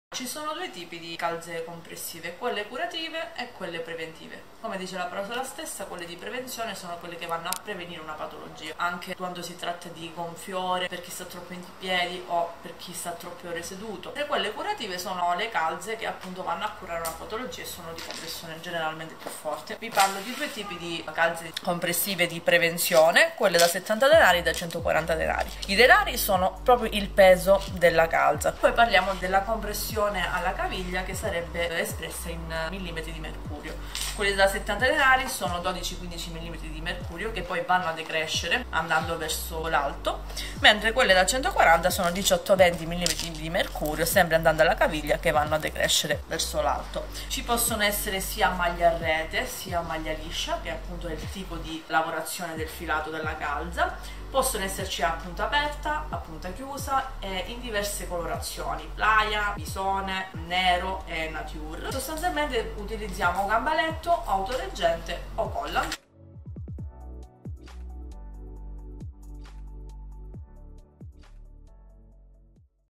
OTR ORTOPEDIA - I nostri prodotti - BLACK WEEK - 50% CALZA MODASANA
Anche le nostre calze Modasana @modasanaofficial sono in 𝑷𝒓𝒐𝒎𝒐... al 50% 🅵🅸🅽🅾 🅰 🆂🅰🅱🅰🆃🅾 🥾👢😍

La Dott.ssa Chiara ci parla di questo articolo!💚